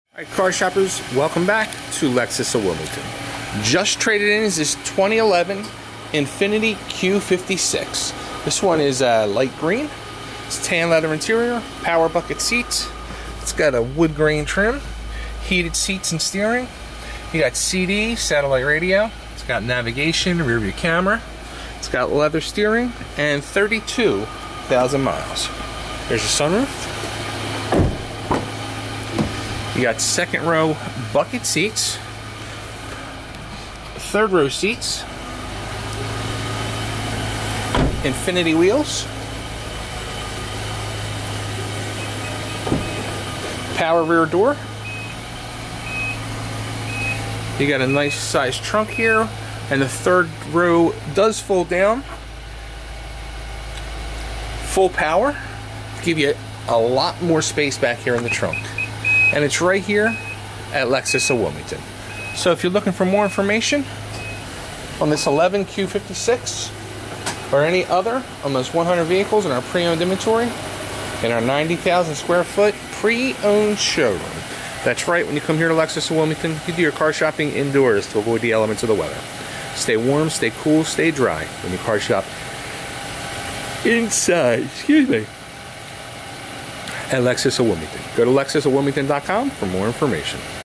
2011 Infiniti Q56 WalkAround Lexus of Wilmington
RUN DON'T WALK, LOW MILES, FREE CARFAX REPORT, NAVIGATION, DEALER SERVICED, MOON ROOF, and ONE OWNER. 4WD and Wheat w/Leather-Appointed Seat Trim or Semi-Aniline Leather Seat Trim.

Want to stretch your purchasing power? Well take a look at this good-looking 2011 Infiniti QX56. Take some of the worry out of buying an used vehicle with this one-owner creampuff. You'll be glad you did! Proudly serving Delaware, Pennsylvania, New Jersey, New York, Maryland and Virginia.